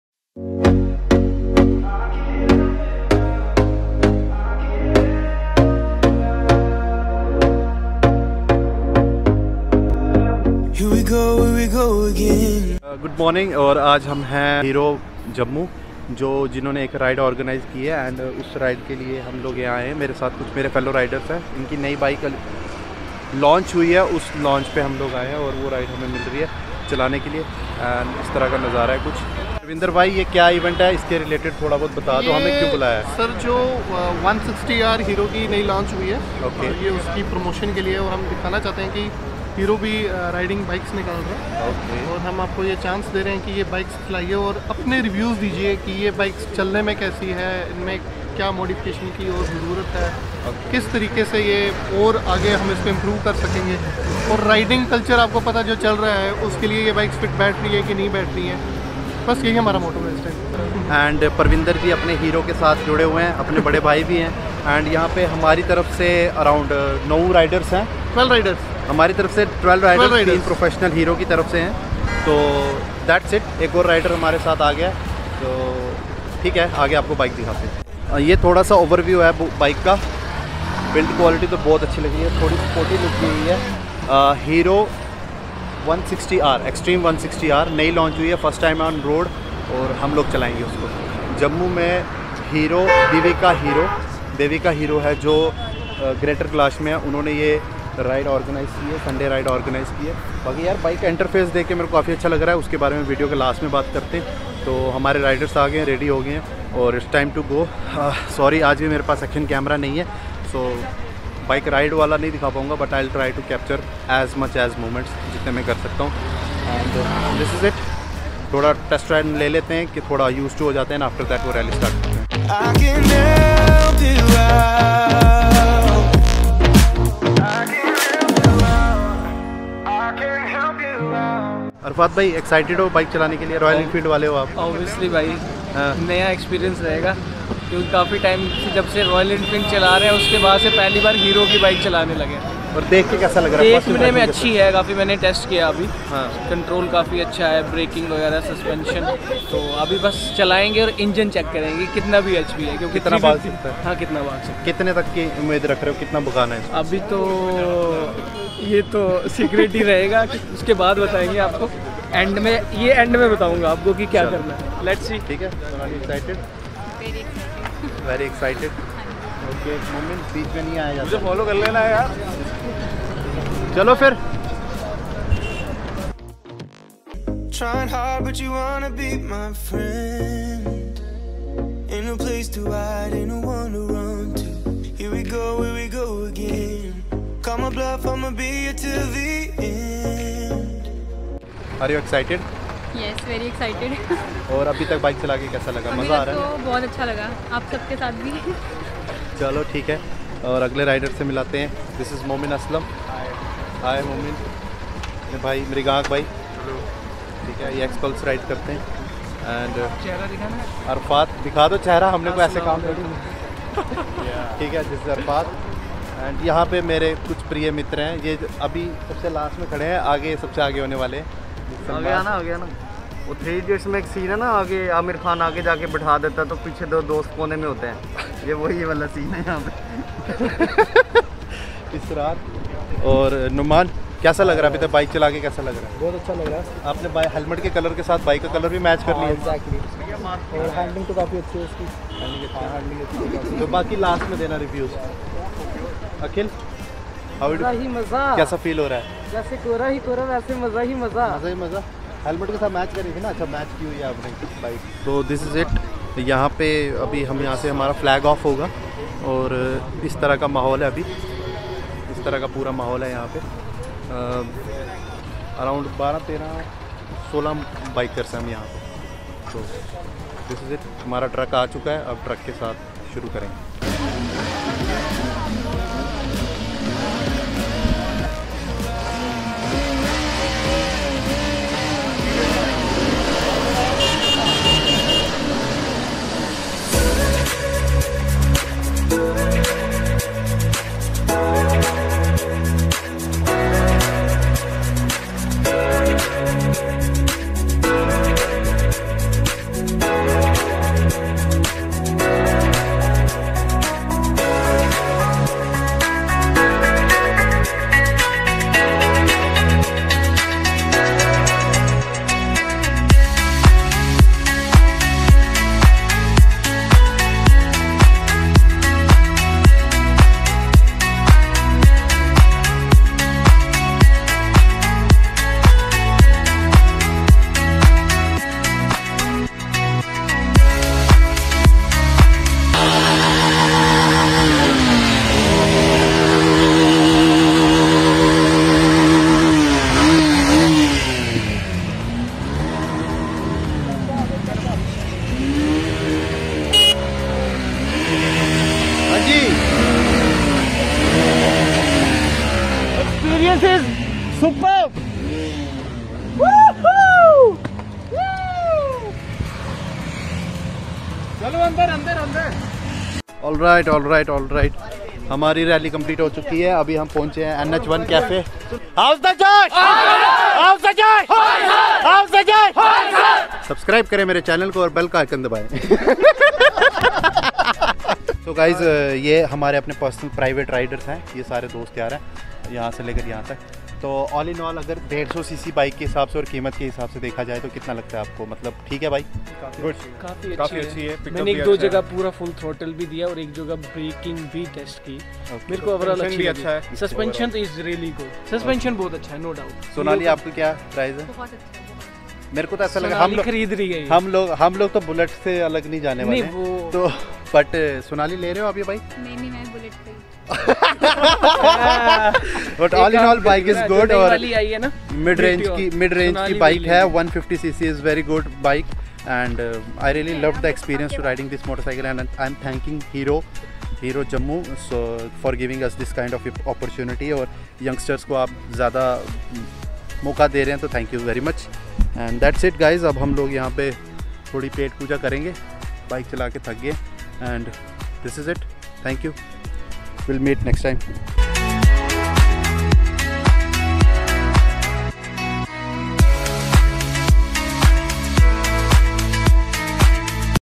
HERO EXTREAME 160R | FIRST EVER HERO RALLY | JAMMU | ASIF ALI
THIS VIDEO IS ABOUT THE FIRST EVER RALLY ORGANISED BY  HERO AT JAMMU FOR SHOWCASING THE NEW LAUNCH OF HERO EXTREAME 160 R.
SHARE YOUR REVIEWS IN THE COMMENT SECTION